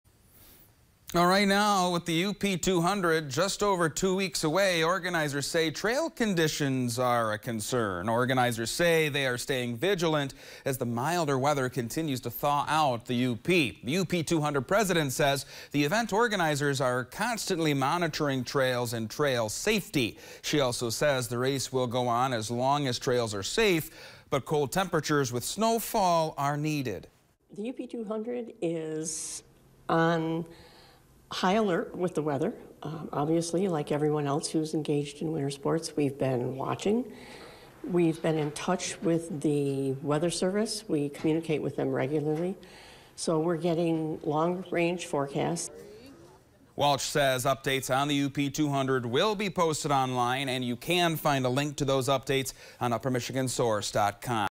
Warm weather puts UP200 organizers on high alert
Organizers say they are staying vigilant as the milder weather continues to thaw out the U.P.